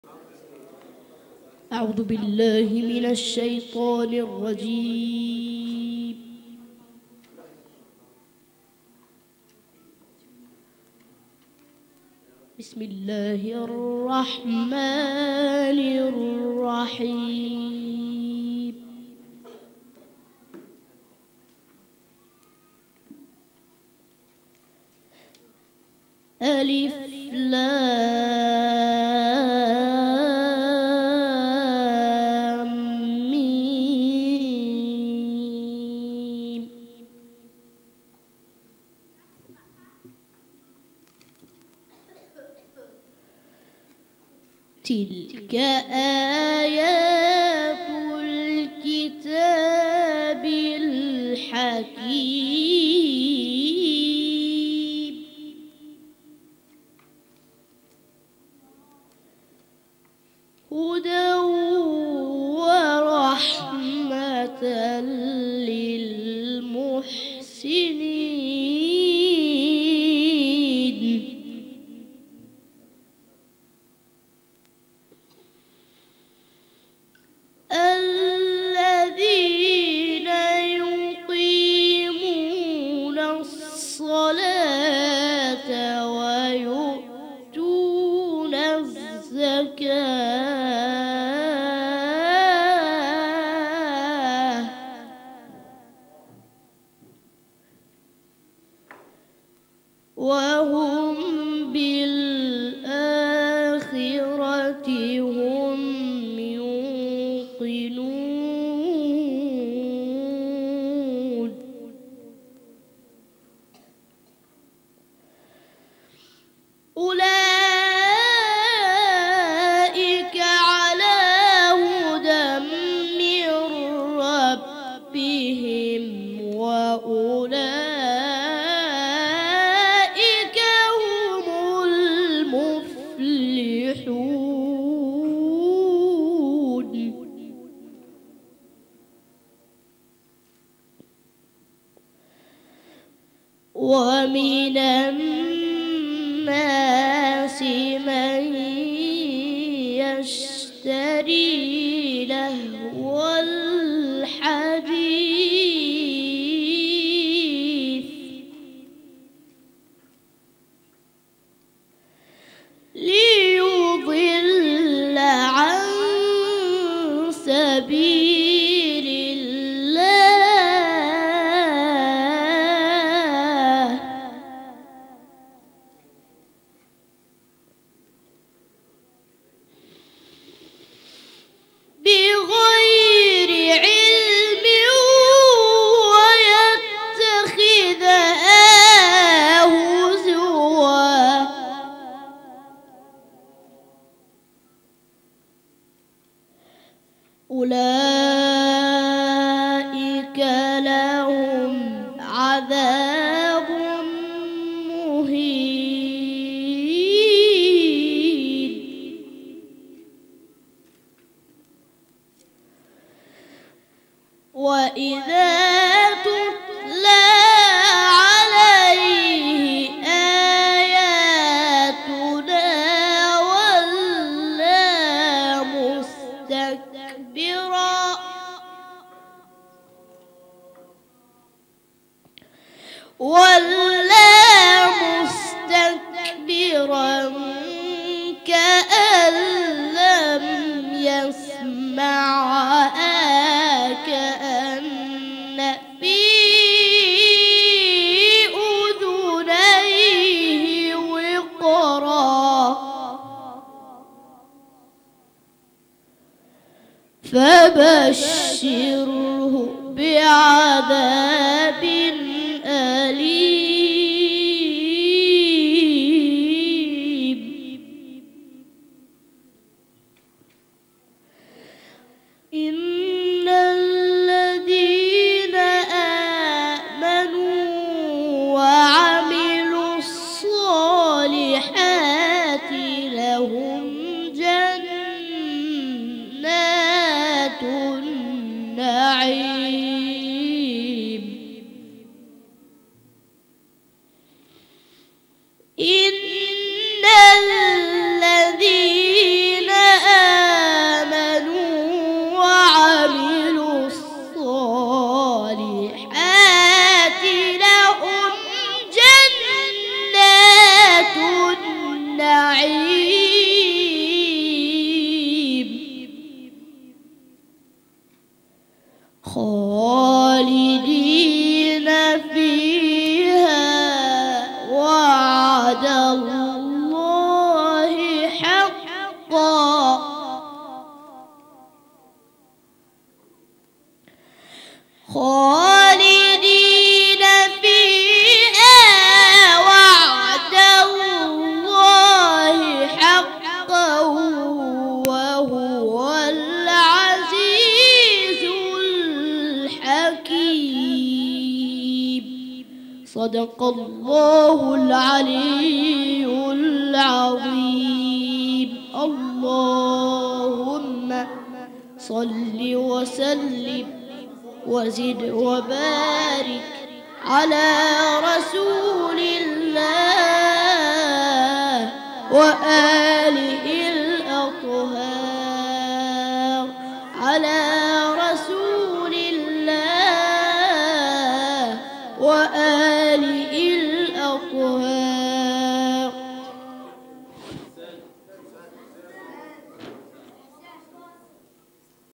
القارىء الطفل باقر محمد البنّي - تلاوة من سورة لقمان
موسم الحسيني الصغير - الليلة الخامسة  ليلة14   صفر 1437هـ - المرخ
تنظيم: هيئة العدل المنتظر الإسلامية، جمعية المرخ الخيرية، مأتم المرخ